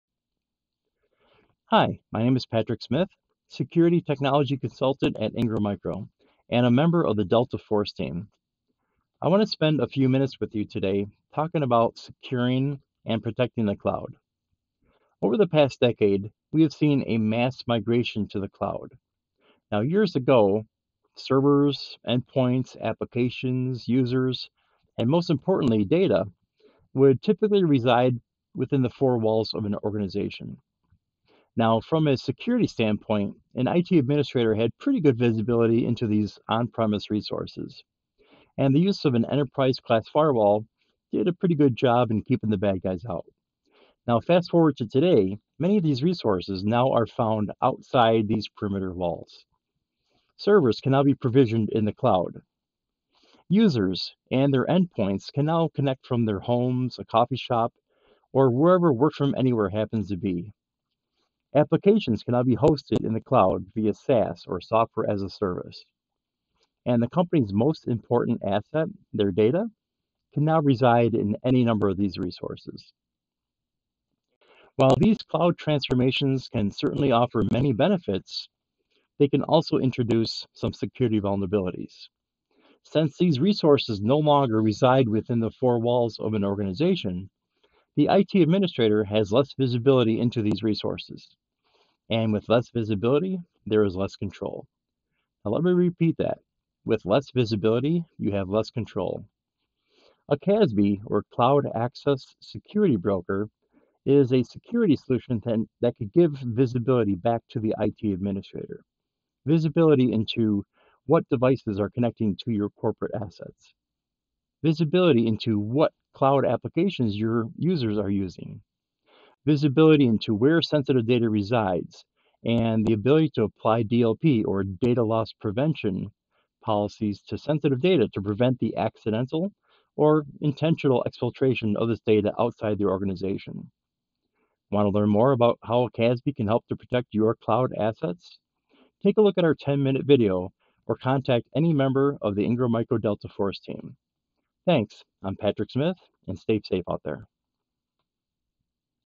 Intro to Secure & Protect the Cloud | Security
Get an introduction to the importance of Securing & Protecting the Cloud from Patrick Smith, Ingram Micro Technology Consultant II.